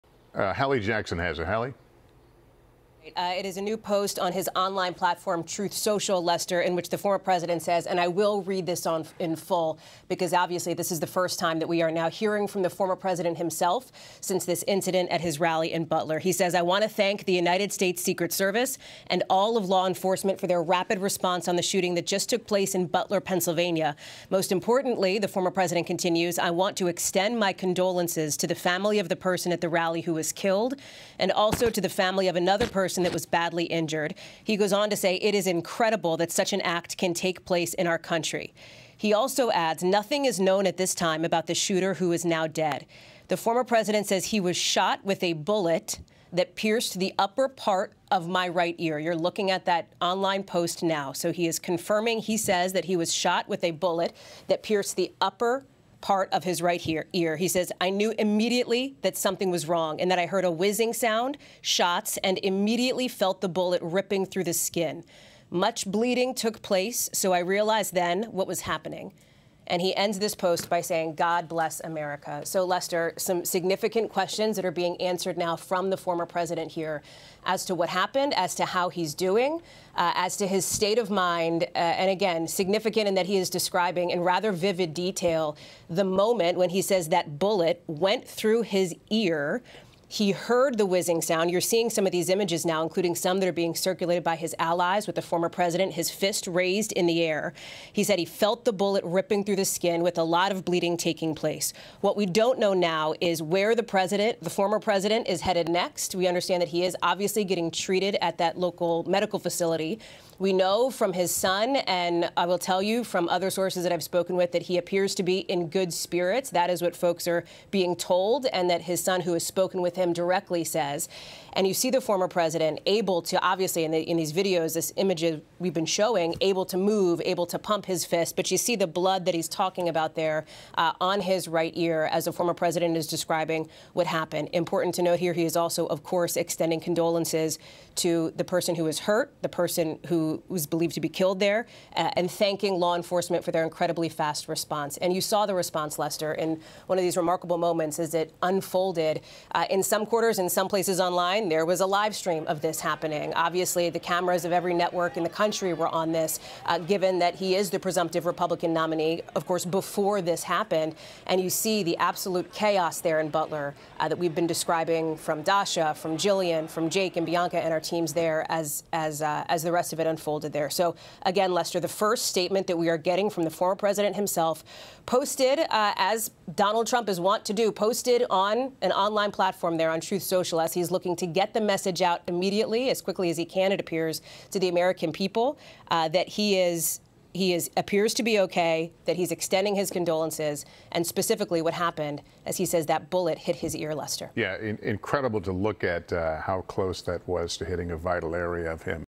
'Felt the bullet ripping through the skin' | Trump statement on rally shooting
Former President Donald Trump released a statement on social media Saturday night following the shooting. Trump appeared to be the target of an assassination attempt as he spoke during a rally in Pennsylvania on Saturday, law enforcement officials said. The former president, his ear covered in blood from what he said was a gunshot, was quickly pulled away by Secret Service agents and his campaign said he was “fine.”

11Alive believes that news shouldn’t be a one-way conversation, but a dialogue with you. Join in, share your thoughts and connect with new perspectives.